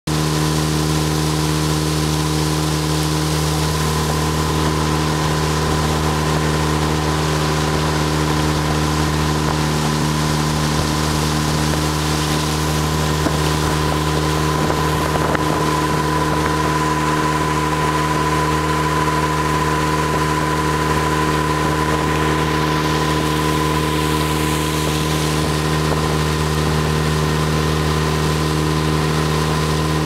Mercury 9.9 4-stroke
Mercury 9.9 4-stroke (from early 2000s) on 1975 Starcraft 14 footer.
About 15 mph.